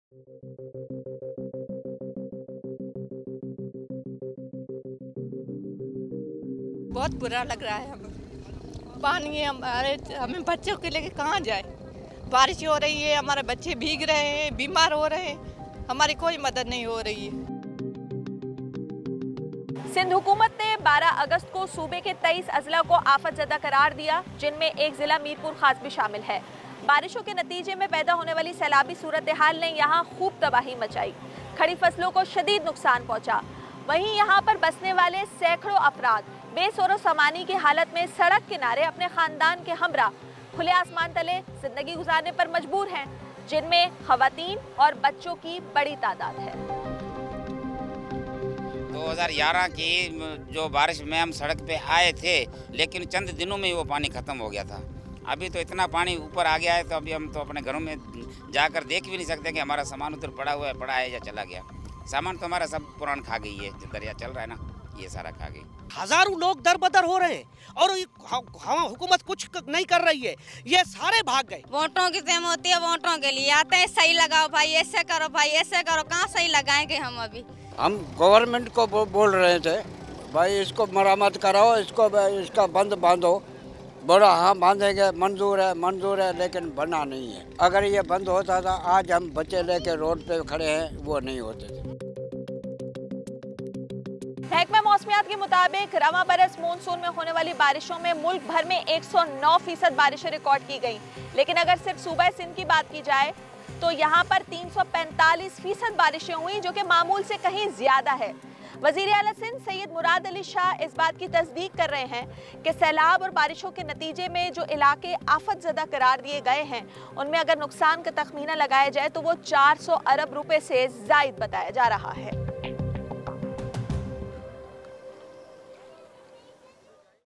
Sindh: Flood situation in district Mir Pur Khas | VOA URDU
"ہزاروں لوگ دربدر ہیں مگر حکومت مدد کو نہیں آ رہی ہے،'' یہ کہنا ہے سندھ کے ضلع میرپور خاص کے سیلاب متاثرہ شخص انوار علی کا جو اس وقت پریشانی کی حالت میں سڑک کنارے رہنے پر مجبور ہیں۔ انوار جیسے کئی سیلاب متاثرین کی کہانی بتارہے ہیں میرپور خاص سے سدرہ ڈار اور خلیل احمد۔